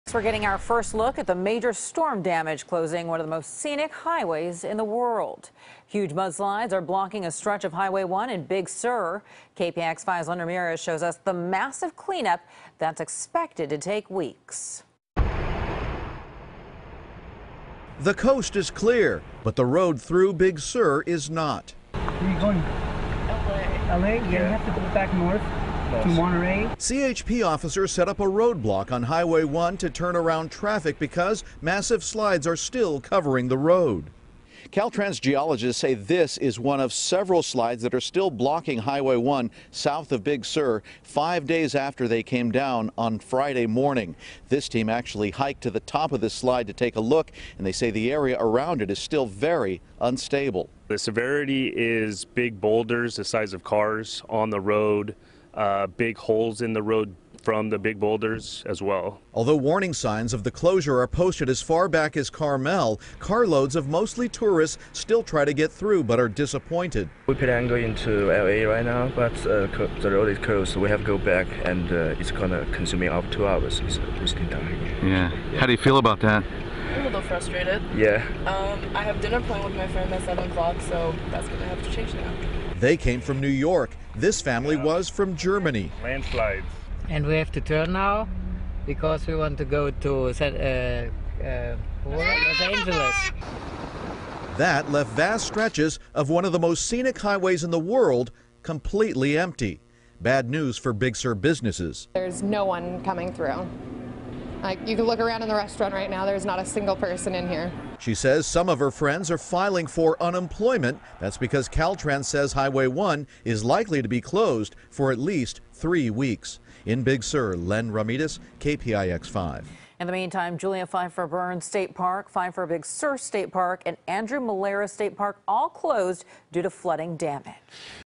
Highway 1 Closed Due To Mudslides Near Big Sur
The California Highway Patrol says Highway 1 could be shut down near Big Sur for three weeks due to mudslides. Len Ramirez reports. (1/24/17)